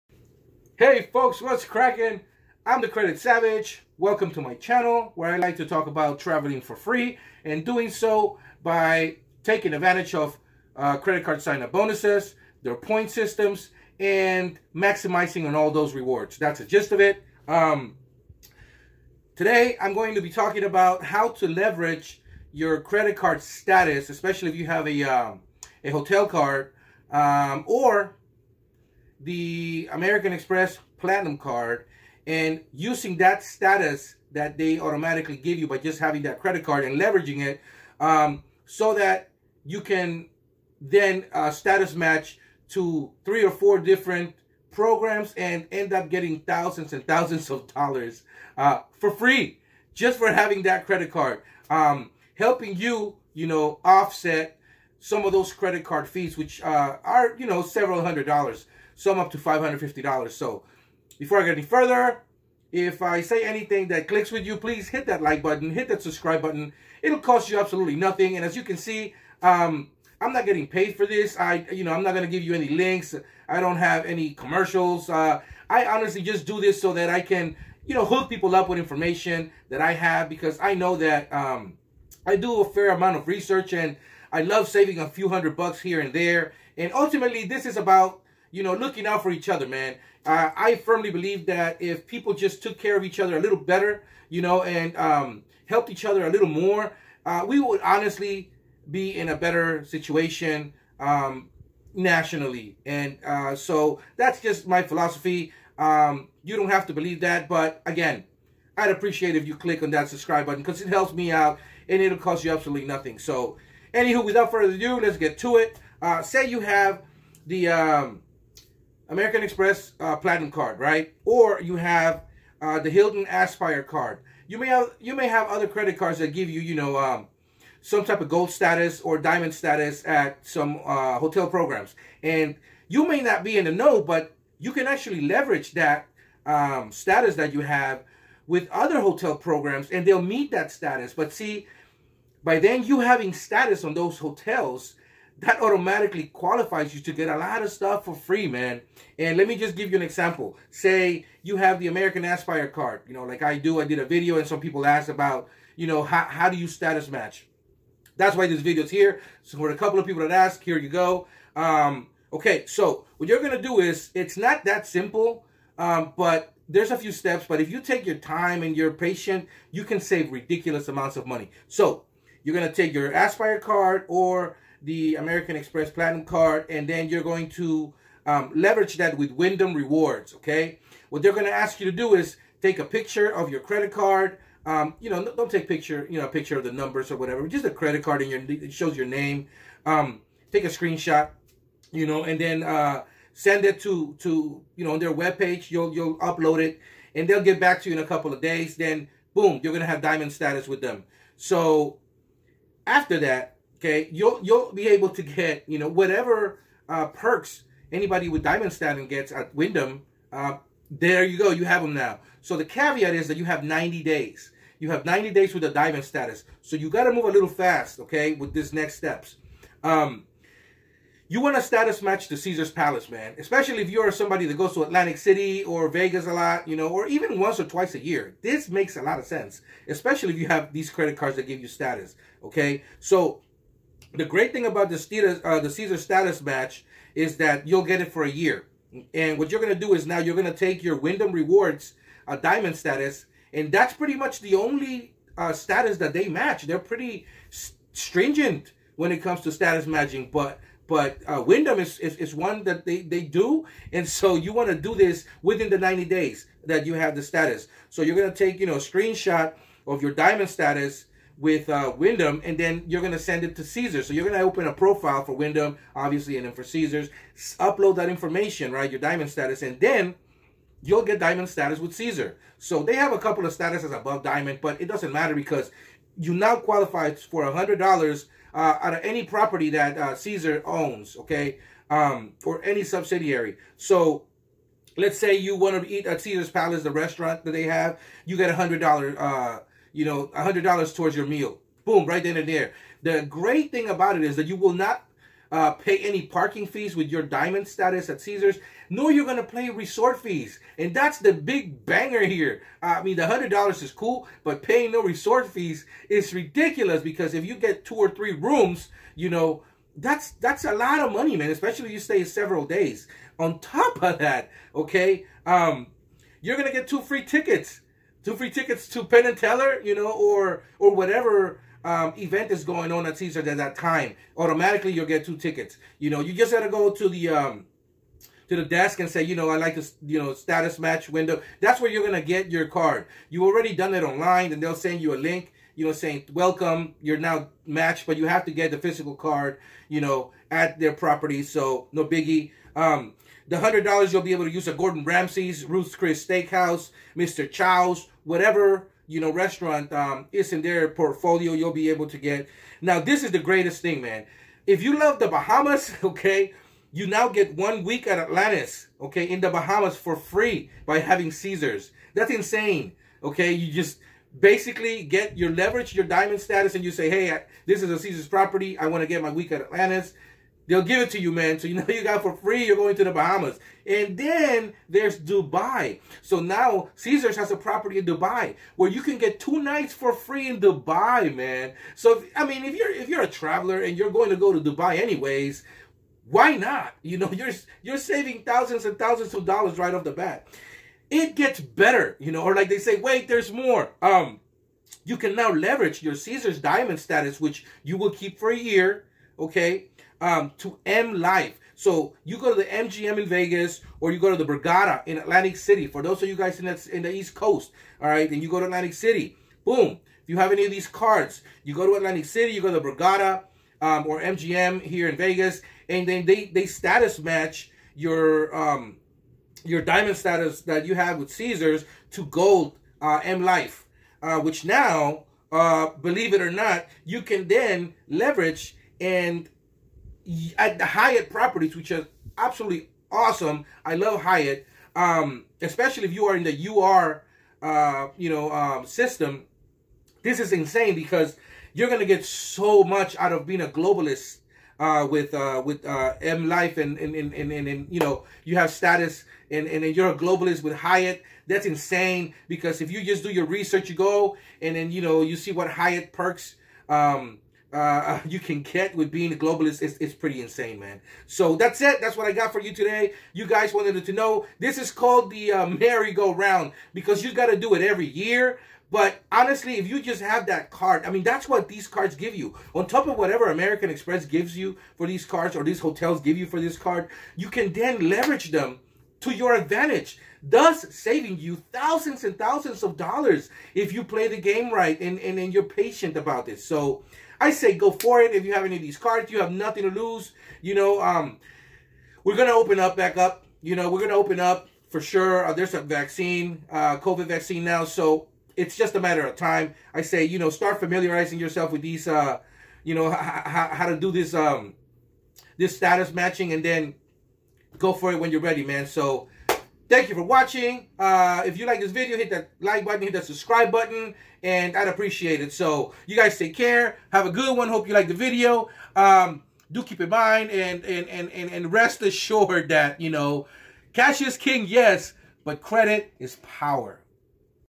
Save THOUSANDS of $ by leveraging your credit card status | AMEX Platinum & Aspire merry-go-round
In this video I'll explain the status match merry-go-round